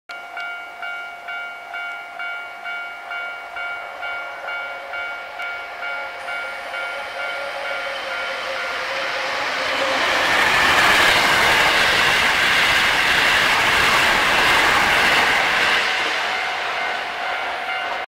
2022.02.27 阪急 9300系9303F 快速急行大阪梅田行き通過 摂津市駅～正雀駅間 正音寺踏切 阪急電鉄 阪急電車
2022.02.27 摂津市駅～正雀駅間 正音寺踏切
9303F 8両編成
(Mc1)9303-(T1)9853-(T2)9873-(T2)9883-(T2)9893-(T1)9863-(M1)9803-(Mc2)9403